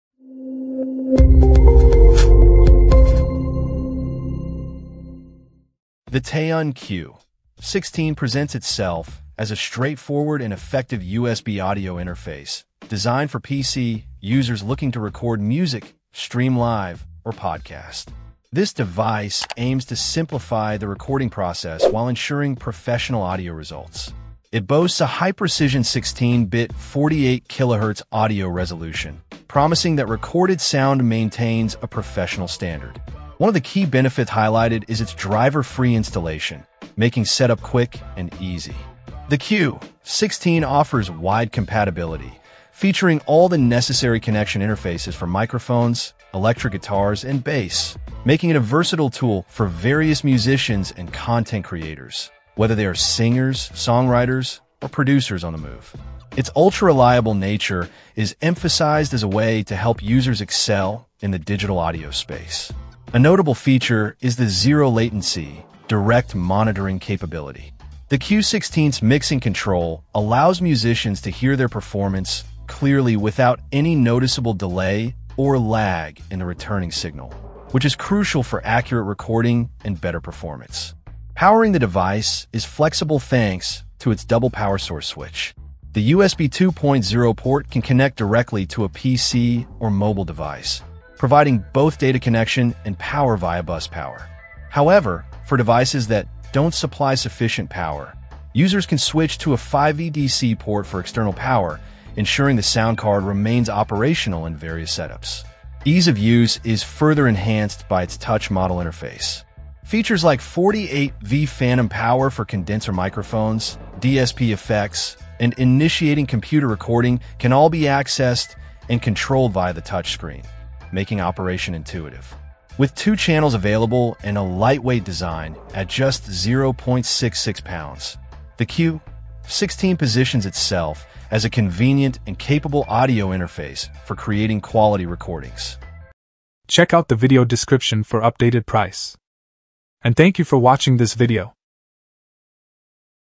ReviewTEYUN Q-16: Easiest Touch Screen Audio Interface for PC?
TEYUN Q-16: Easiest Touch Screen Audio Interface for PC?

0:00 Intro
0:06 Review

Things that we mentioned in this video:
USB Audio Interface for : B0CDBXSN9Z